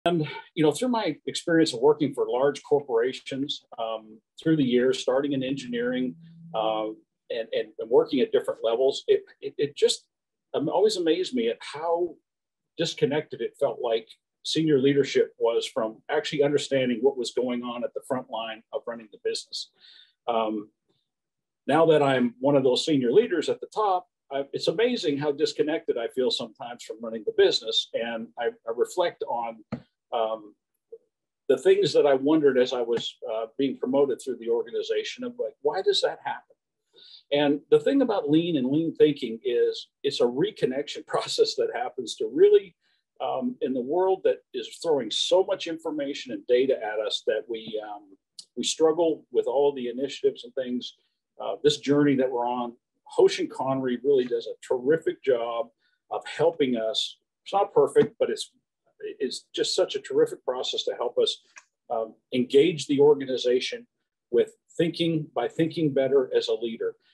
Why Hoshin Kanri?
Bill MacPherson, managing director of Mercer Celgar, a division of Mercer International, describes the problems he was trying to solve when he launched his hoshin journey -- and why lean thinking in general and hoshin kanri, in particular, helps solve them. Read and learn from his story, Breaking the Cycle of Ineffective Planning and Execution

Audio is from a webinar produced by The Ohio State University Fisher College of Business Center for Operational Excellence